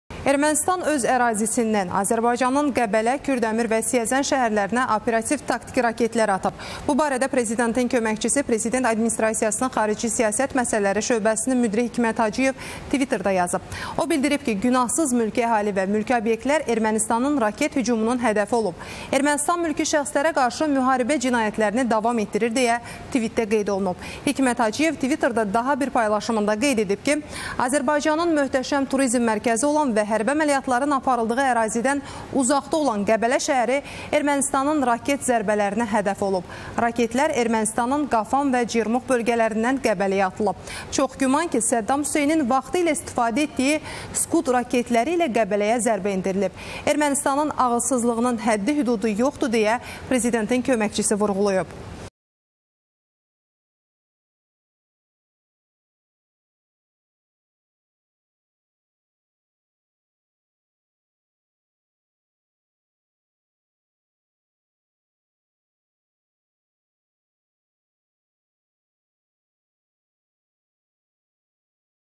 Hikmət Hacıyev tviterdə daha bir paylaşım edib (22.10.2020)
Ermənistan öz ərazisindən Azərbaycanın Qəbələ, Kürdəmir və Siyəzən şəhərlərinə operativ taktiki raketlər atıb. Bu barədə Prezidentin köməkçisi, Prezident Administrasiyasının Xarici Siyasət Məsələləri Şöbəsinin müdiri Hikmət Hacıyev “Twitter”də yazıb. O, bildirib ki, günahsız mülki əhali və mülki obyektlər Ermənistanın raket hücumunun hədəfi olub: “Ermənistan mülki şəxslərə qarşı müharibə cinayətlərini davam etdirir”.-deyə tvitdə qeyd olunub. Hikmət Hacıyev tviterdə daha bir paylaşımında qeyd edib ki,  Azərbaycanın möhtəşəm turizm mərkəzi olan və hərbi əməliyyatların aparıldığı ərazidən uzaqda olan Qəbələ şəhəri Ermənistanın raket zərbələrinə hədəf olub. “Raketlər Ermənistanın Qafan və Cermux bölgələrindən Qəbələyə atılıb. Çox güman ki, Səddam Hüseynin vaxtilə istifadə etdiyi SCUD raketləri ilə Qəbələyə zərbə endirilib. Ermənistanın ağılsızlığının həddi-hüdudu yoxdur"-deyə Prezidentin köməkçisi vurğulayıb.